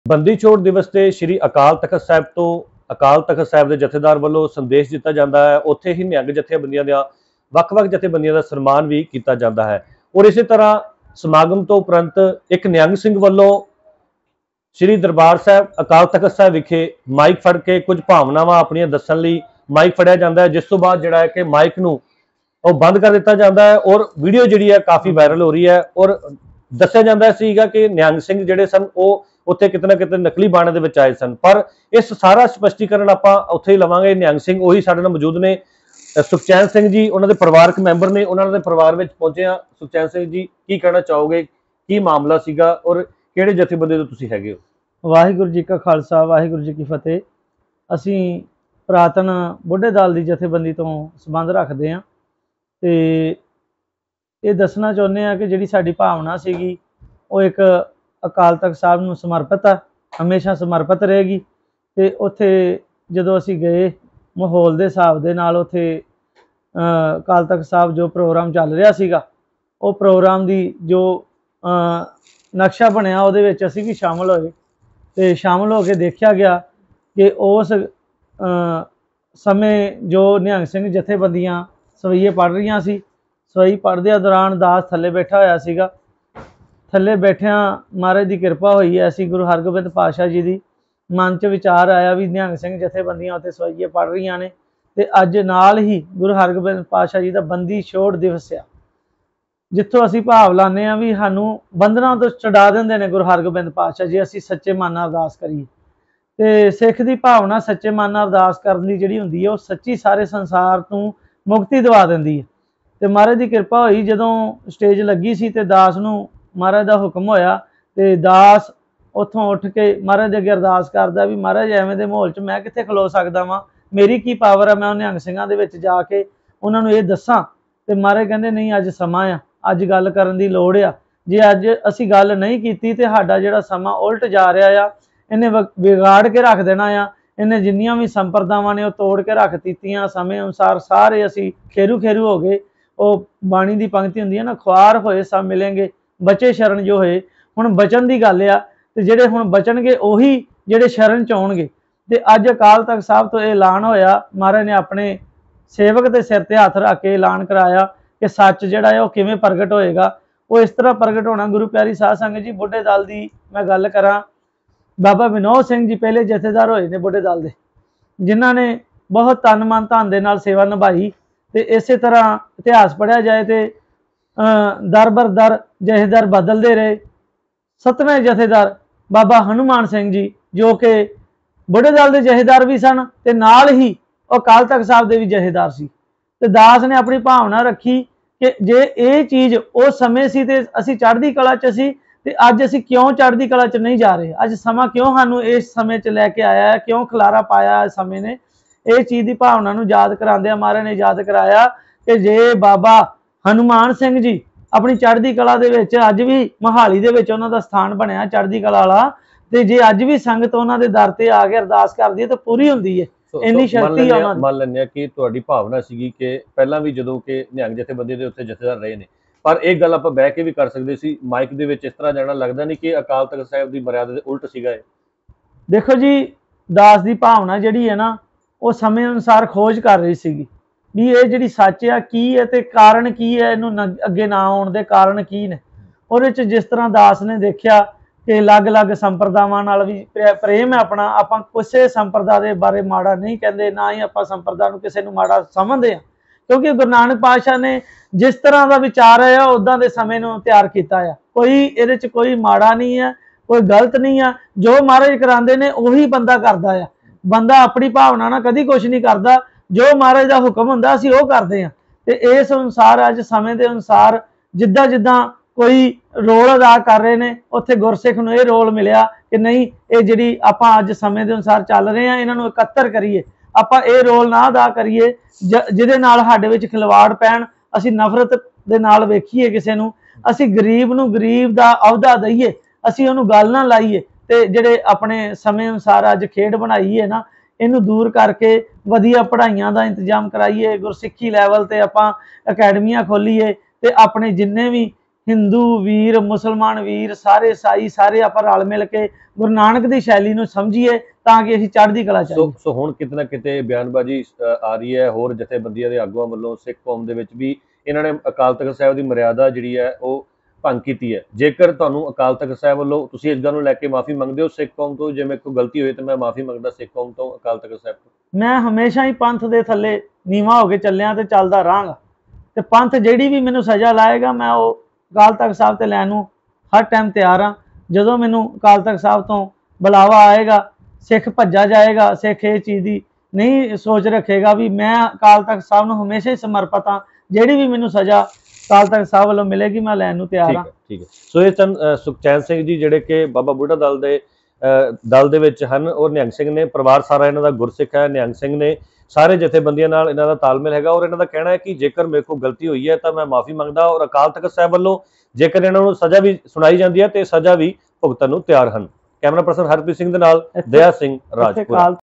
ਸ੍ਰੀ ਅਕਾਲ ਤਖ਼ਤ ਸਾਹਿਬ 'ਤੇ ਮਾਈਕ 'ਤੇ ਬੋਲਣ ਵਾਲੇ ਨਿਹੰਗ ਦਾ ਪਹਿਲਾਂ ਬਿਆਨ
ਕੈਮਰੇ ਆਪਣੇ ਰੱਖਿਆ ਆਪਣਾ ਸਪਸ਼ਟੀਕਰਨ